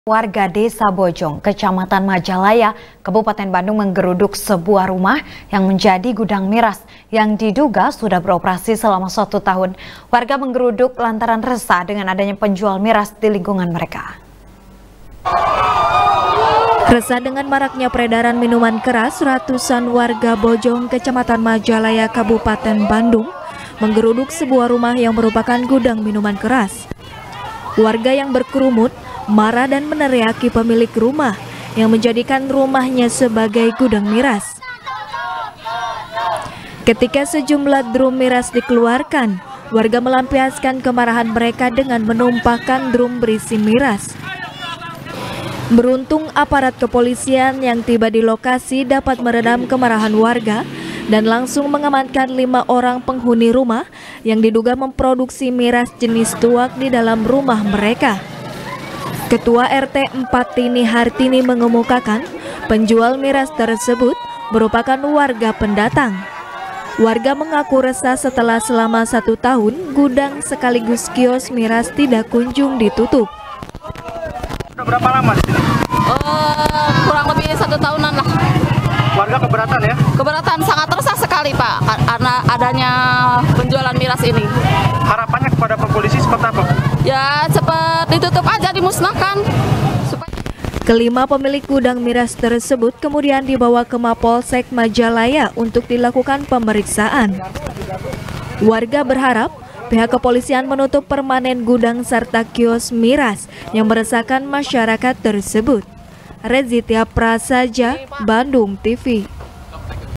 Ratusan Warga Geruduk Gudang Miras Di Majalaya | SBR | BANDUNGTV
SBR | BANDUNGTV | NIKEN PRATIWI | Ratusan Warga Desa Bojong,Kecamatan Majalaya,Kabupaten Bandung,Menggeruduk Sebuah Rumah,Yang Menjadi Gudang Miras,Yang Diduga Sudah Beroperasi Selama Satu Tahun.Warga Menggeruduk,Lantaran Resah Dengan Adanya Penjual Miras,Di Lingkungan Mereka.